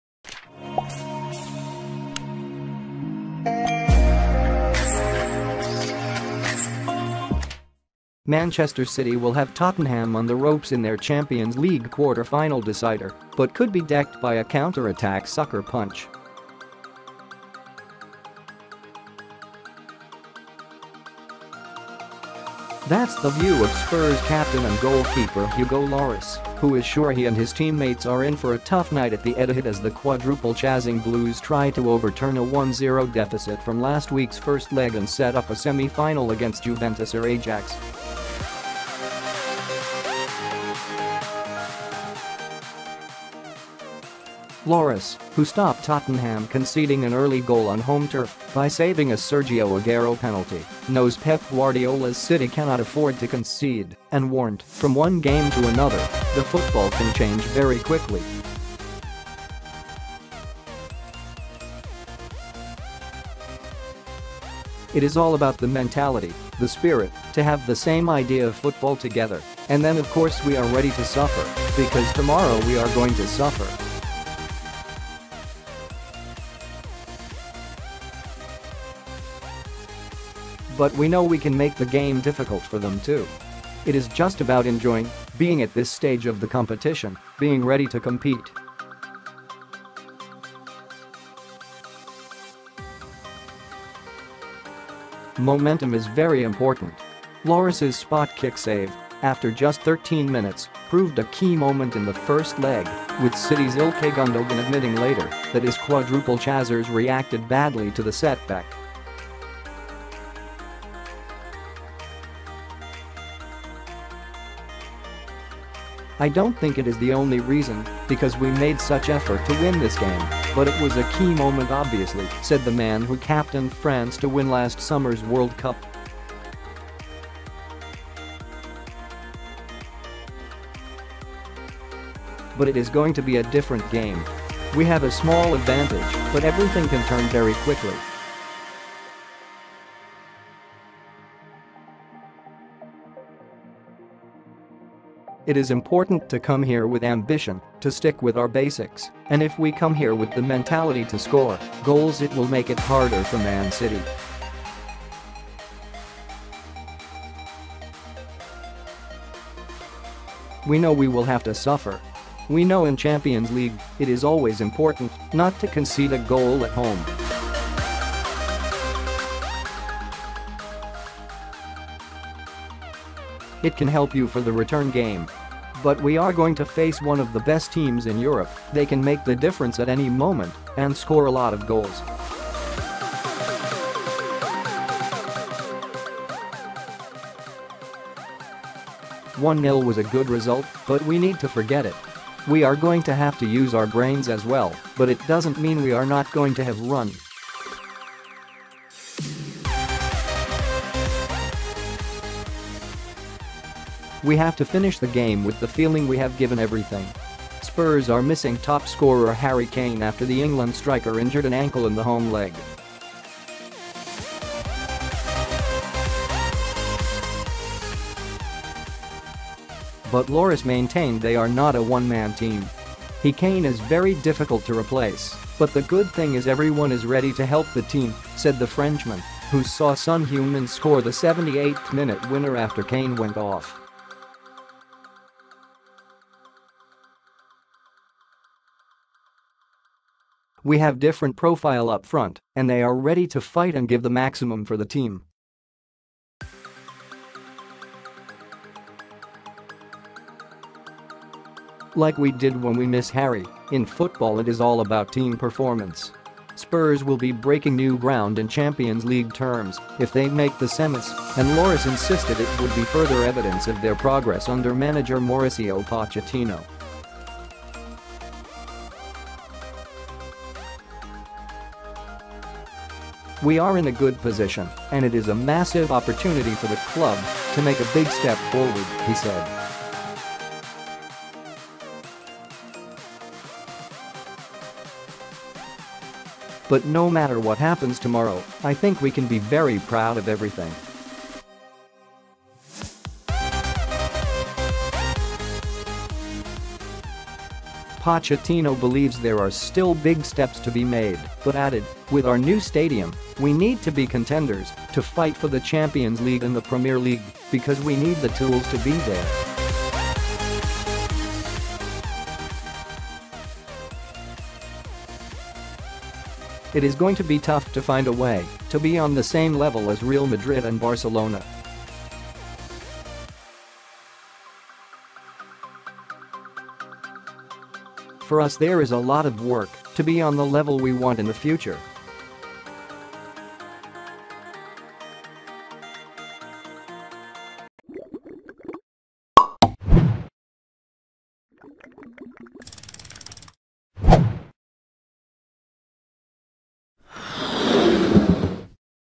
Why Hugo Lloris is promising Tottenham pain in Man City Champions League decider Man City News:
Manchester City WILL have Tottenham on the ropes in their Champions League quarter-final decider — but could be decked by a counter-attack sucker-punch.   That's the view of Spurs captain and goalkeeper Hugo Lloris, who is sure he and his team-mates are in for a tough night at the Etihad as the Quadruple-chasing Blues try to overturn a 1-0 deficit from last week's first leg and set up a semi-final against Juventus or Ajax.   Lloris, who stopped Tottenham conceding an early goal on home turf by saving a Sergio Aguero penalty, knows Pep Guardiola's City cannot afford to concede, and warned: “From one game to another, the football can change very quickly.   “It is all about the mentality, the spirit, to have the same idea of football together and then of course we are ready to suffer because tomorrow we are going to suffer.   “But we know we can make the game difficult for them, too. It is just about enjoying being at this stage of the competition, being ready to compete. Momentum is very important.”  Lloris’s spot-kick save, after just 13 minutes, proved a key moment in the first leg, with City's Ilkay Gundogan admitting later that his Quadruple chasers reacted badly to the setback.  “I don’t think it is the only reason because we made such effort to win this game but it was a key moment obviously,” said the man who captained France to win last summer's World Cup. “But it is going to be a different game. We have a small advantage but everything can turn very quickly.  “It is important to come here with ambition, to stick with our basics and if we come here with the mentality to score goals it will make it harder for Man City.    “We know we will have to suffer.  “We know in Champions League, it is always important not to concede a goal at home. It can help you for the return game. But we are going to face one of the best teams in Europe, they can make the difference at any moment and score a lot of goals.  “One-nil was a good result, but we need to forget it. We are going to have to use our brains as well but it doesn’t mean we are not going to have run. We have to finish the game with the feeling we have given everything.”  Spurs are missing top-scorer Harry Kane after the England striker injured an ankle in the home leg. But Lloris maintained they are not a one-man team.  “He [Kane] is very difficult to replace, but the good thing is everyone is ready to help the team," said the Frenchman, who saw Son Heung-min score the 78th minute winner after Kane went off.    “We have different profile up front and they are ready to fight and give the maximum for the team. Like we did when we miss Harry, in football it is all about team performance.”  Spurs will be breaking new ground in Champions League terms if they make the semis, and Lloris insisted it would be further evidence of their progress under manager Mauricio Pochettino.  “We are in a good position and it is a massive opportunity for the club to make a big step forward,” he said. “But no matte